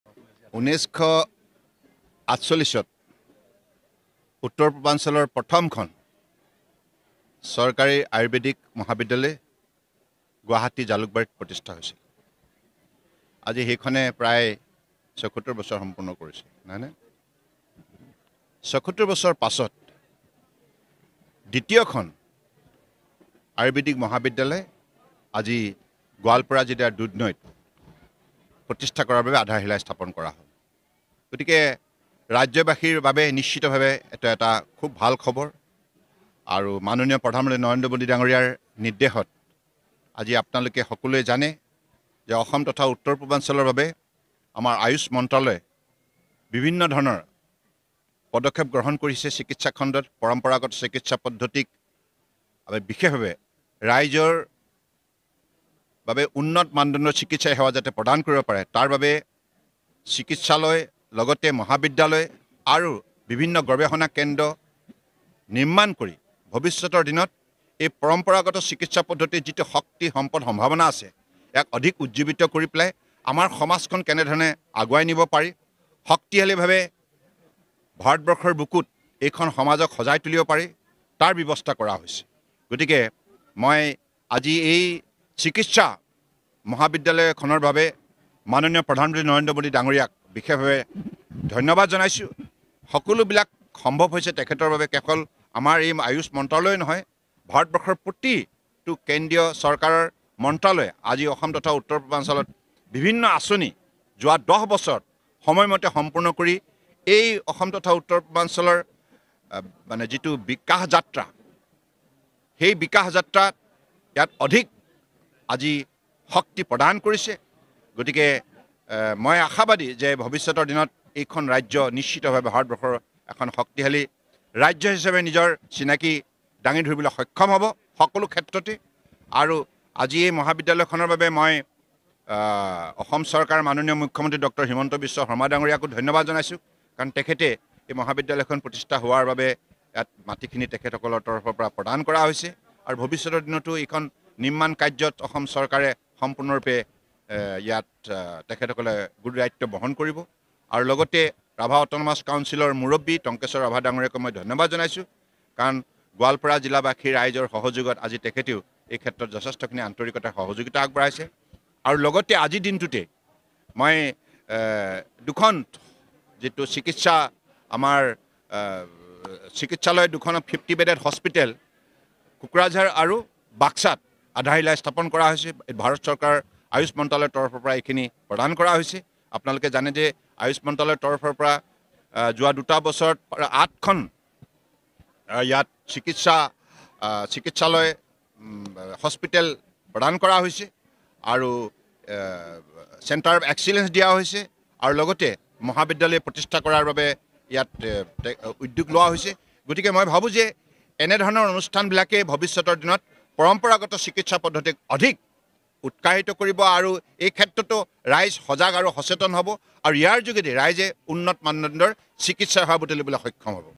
Ayurvedic College In Dudhnoi | Sarbananda Sonowal | দুধনৈত আয়ুৰ্বেদিক মহাবিদ্যালয়ৰ শিলান্যাস | N18V
লগতে চাওঁক দিনটোৰ বহু গুৰুত্বপূৰ্ণ খবৰ |